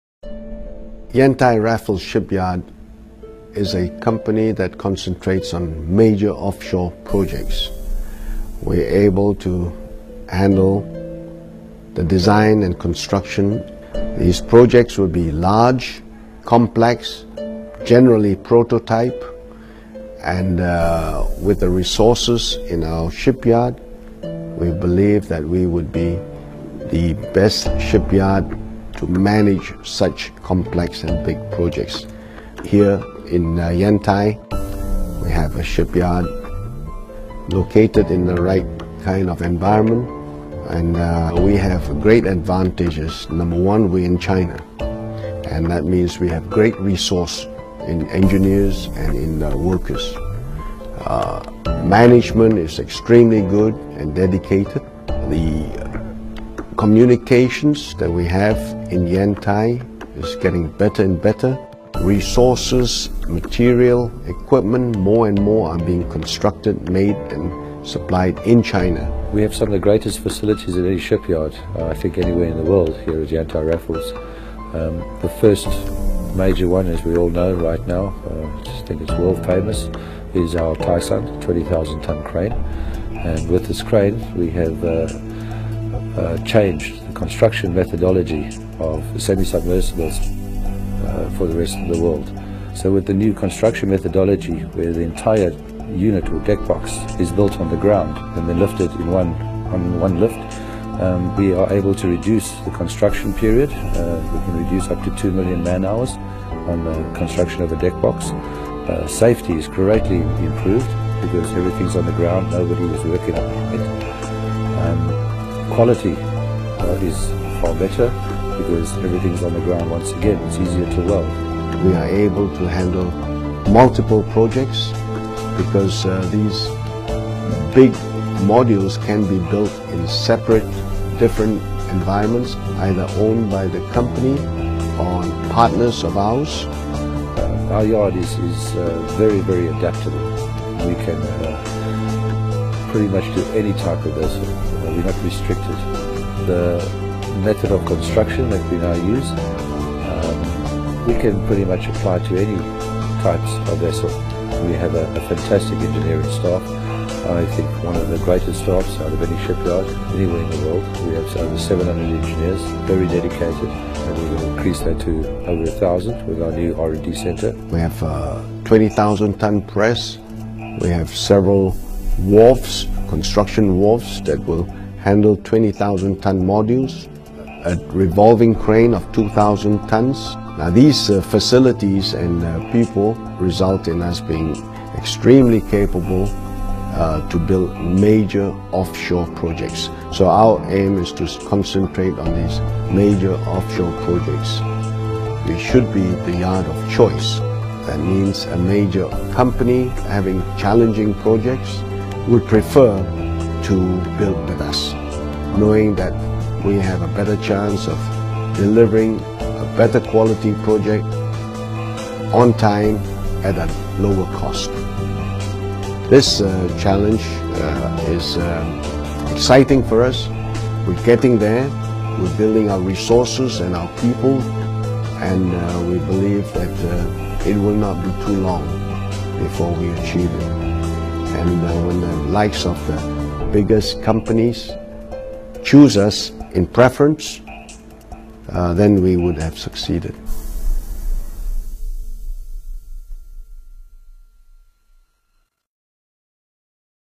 烟台莱福士船厂 (CIMC Raffles Offshore) World's Largest Crane
World's Largest Crane
Taisun, the world's largest gantry crane, with a lifting capacity of 20,000 metric tonnes, has taken over 7 years to plan and an additional 12 months for the preparation of its design framework.

The crane, capable of muscling up to 10,000 cars in one lift, allows the mating of an entire outfitted deck box of a semi-submersible rig onto its hull/pontoons in one single operation. The shipyard states that this will reduce work hazards at high altitudes and in the open sea. The lifting of the complete deck box reduces man-hours by as much as half